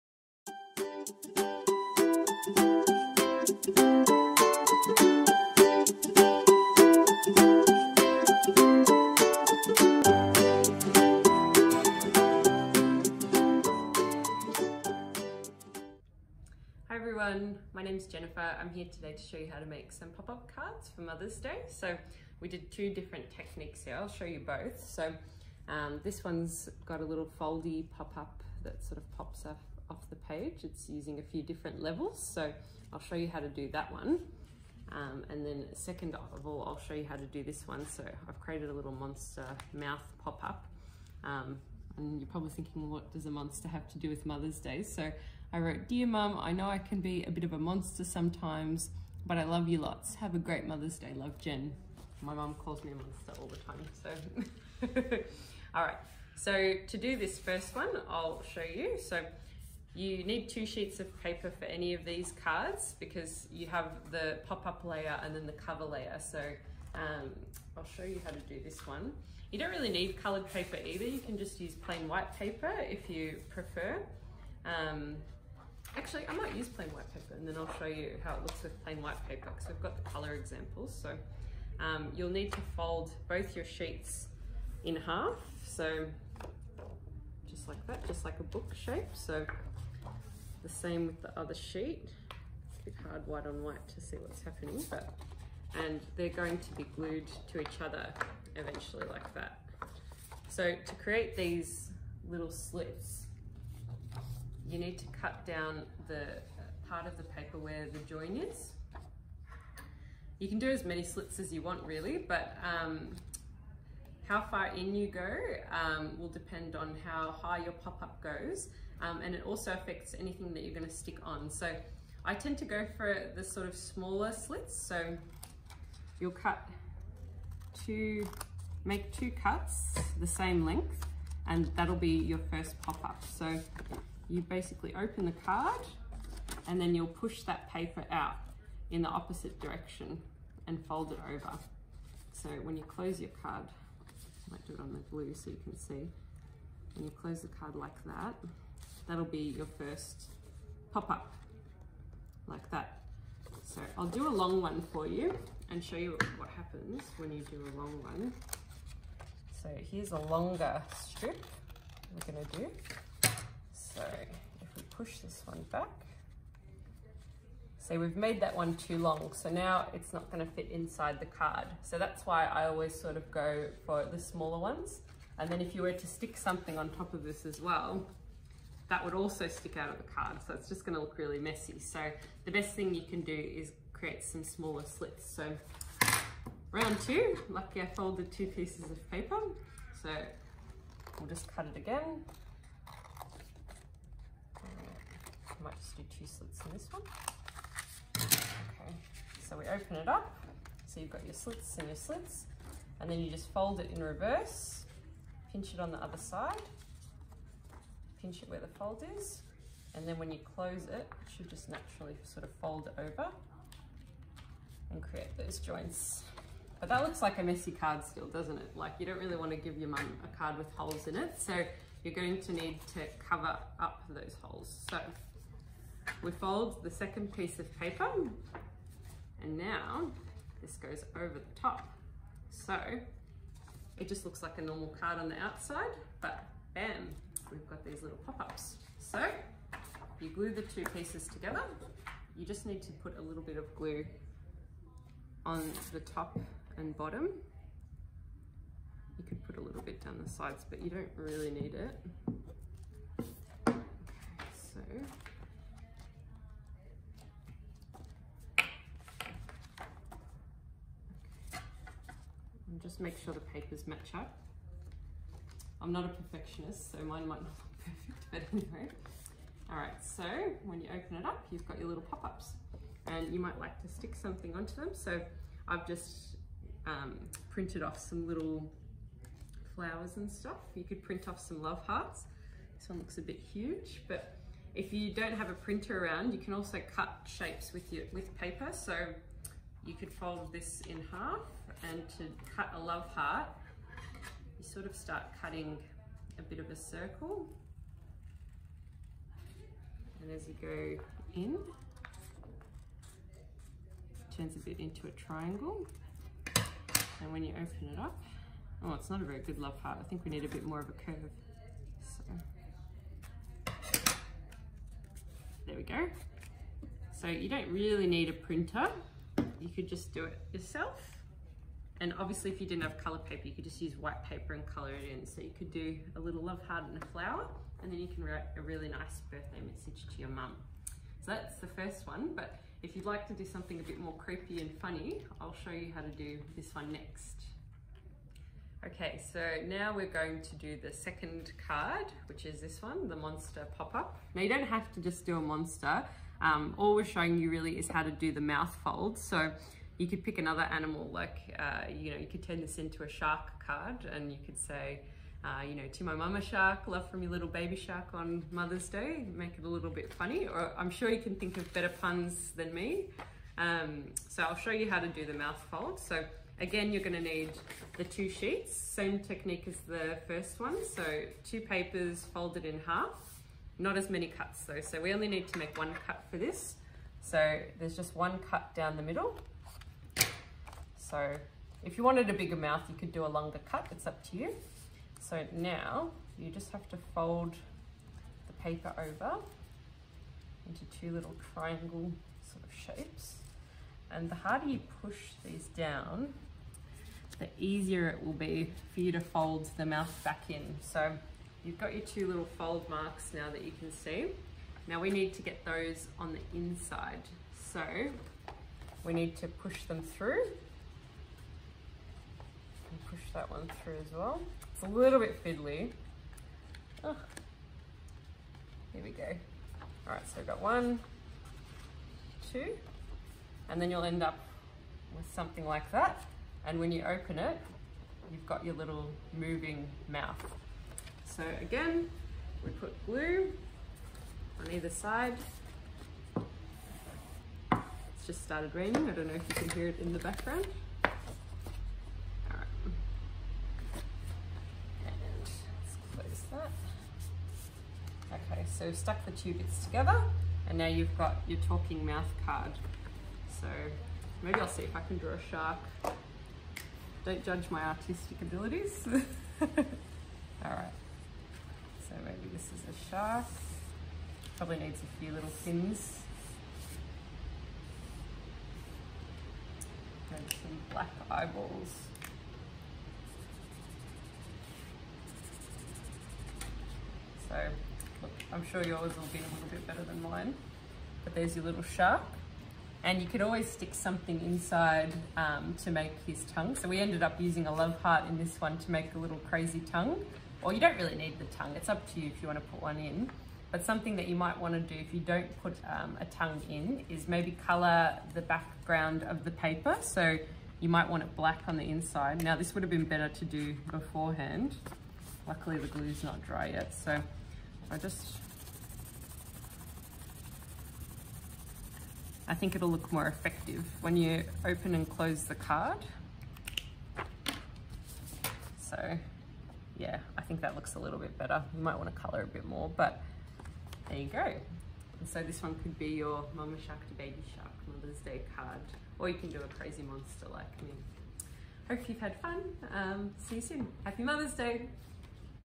Mother's day card with Jen
It’s Mother’s Day this weekend! Join Jennifer from Deer Park Library and learn how to create 2 different pop-up cards and spoil your Mum or Grandma this Mother’s Day!”